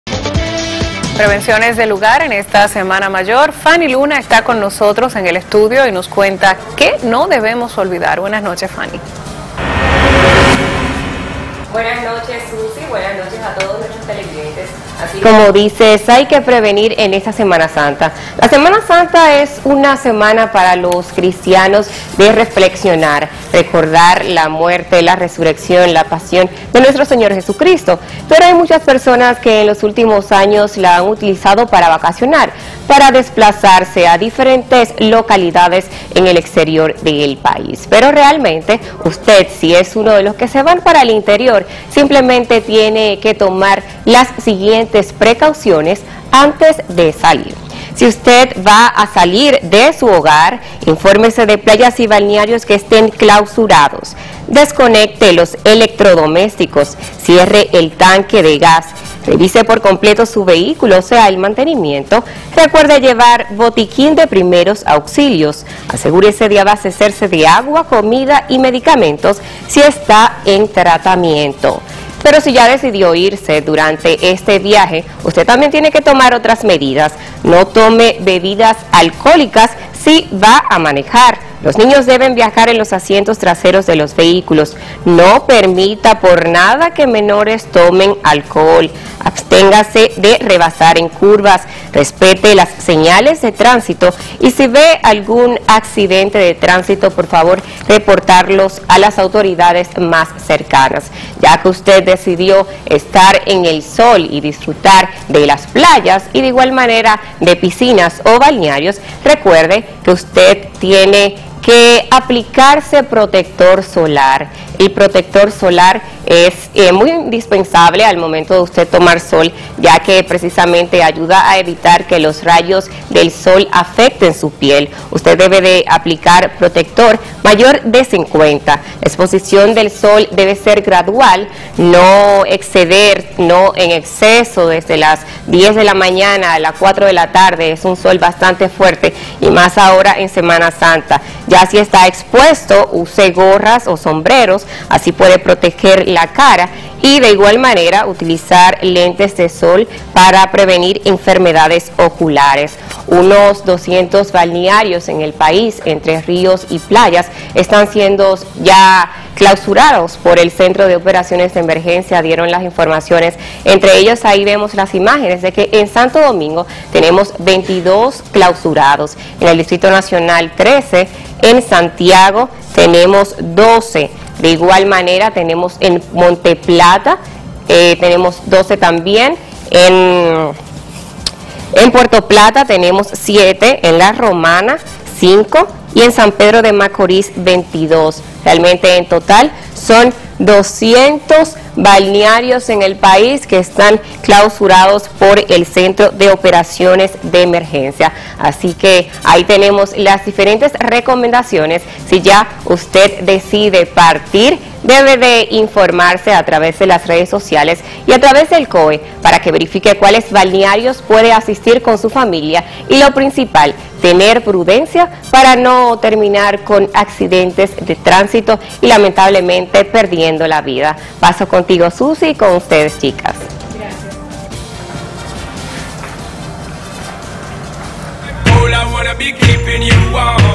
Fany Luna Nos presentó medidas de prevención para semana santa 2016
medidas de prevención para el asuento de la semana mayor 2016.